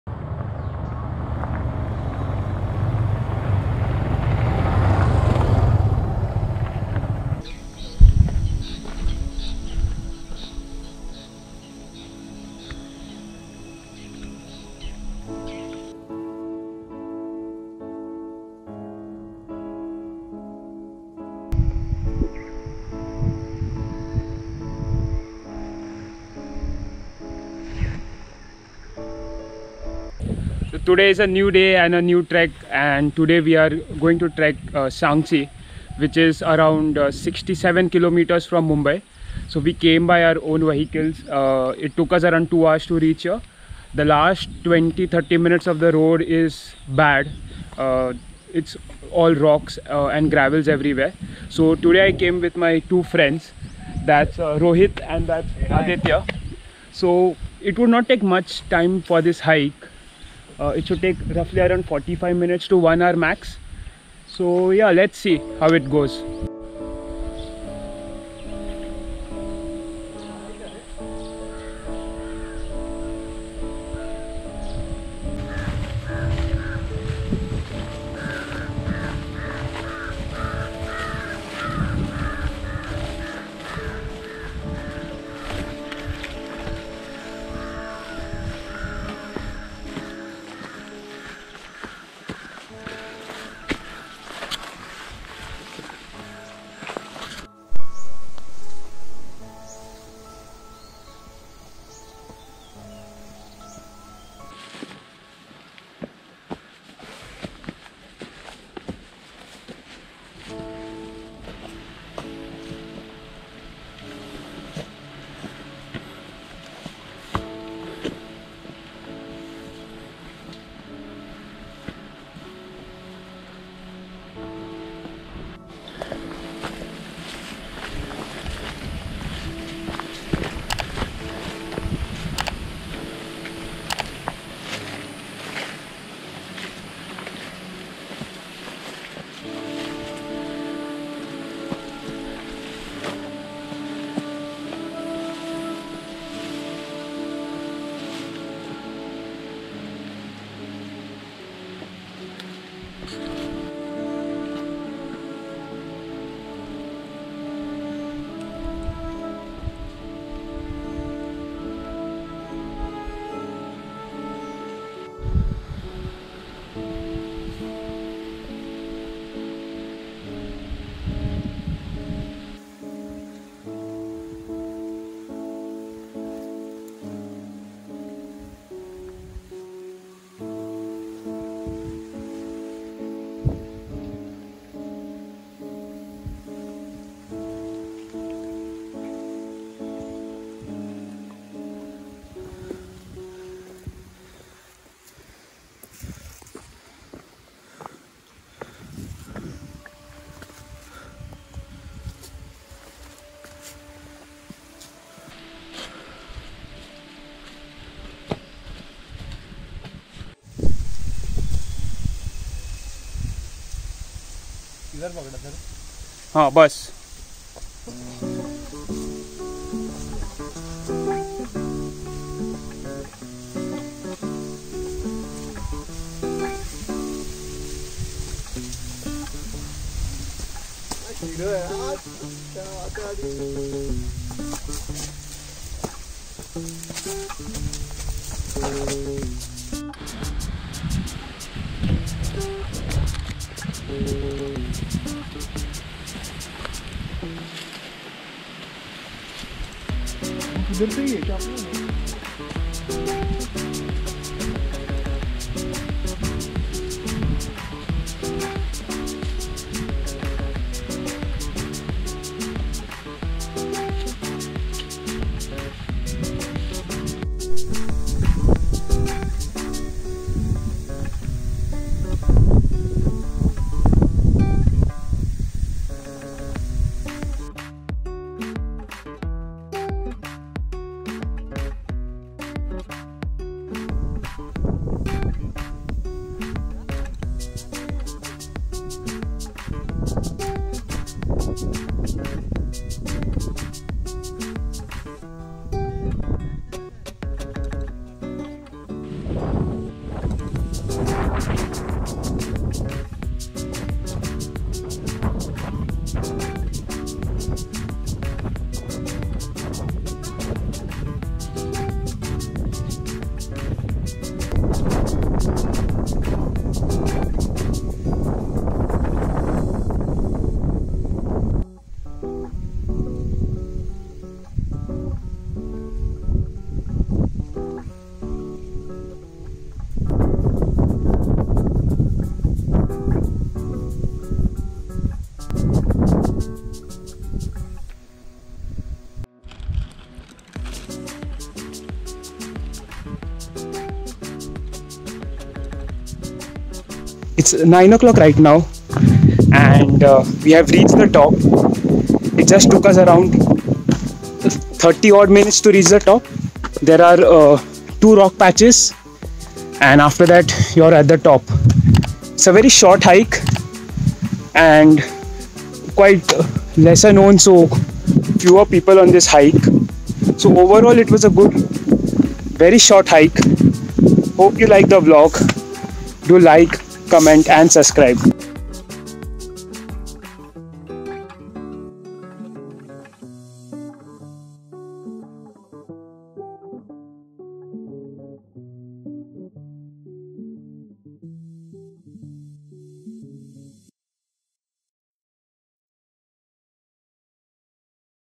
Sankshi Fort | Western Ghats | Unknown Trek Route
Sankshi Fort trek is a  short 30 min hike which can be done in a day. It starts from the village of Nidhavali which is a small hamlet off the Mumbai - Goa Highway , before Pen . One can see forts like Manikgad and Karnala from it .